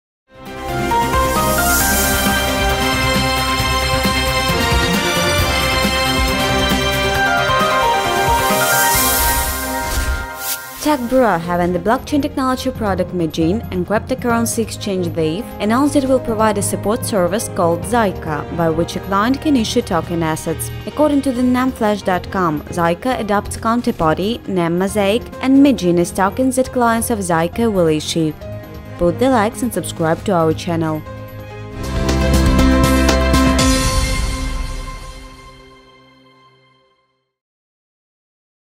KCN Digest: Tech Bureau & Zaif to provide “Zaica” service
Tech Bureau, having the blockchain technology product “Mijin” and crypto-currency exchange “Zaif”, announced it will provide a support service called “Zaica”, by which a client can issue token-assets. According to the Nemflash.com, Zaica adopts Counterparty, NEM Mosaic, and mijin as tokens that clients of Zaica will issue.